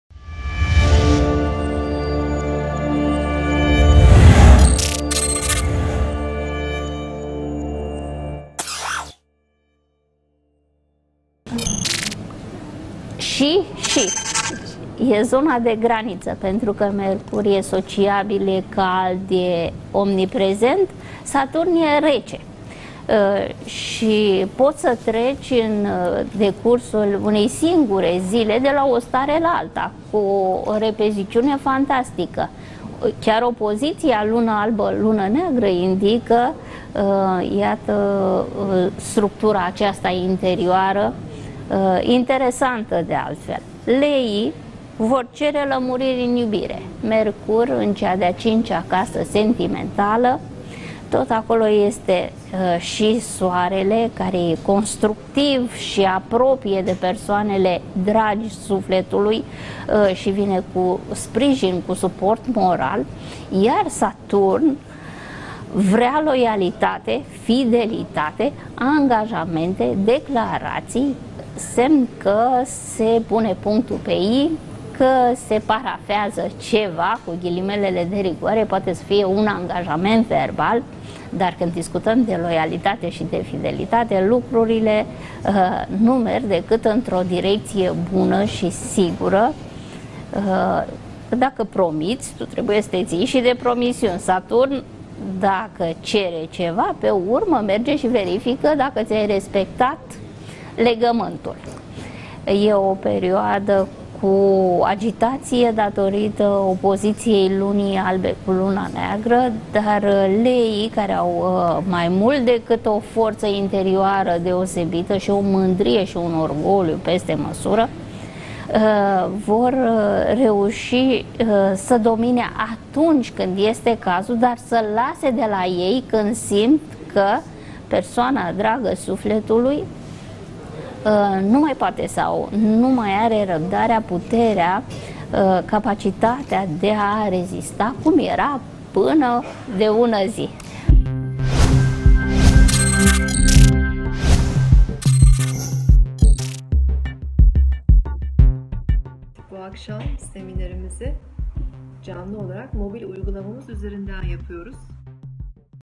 Leu - horoscopul săptămânii 23-29 Noiembrie
Leu- Iubirea cere lămuriri (Me), implicare (So), seriozitate și fidelitate (Sat). ,,Cine nu mai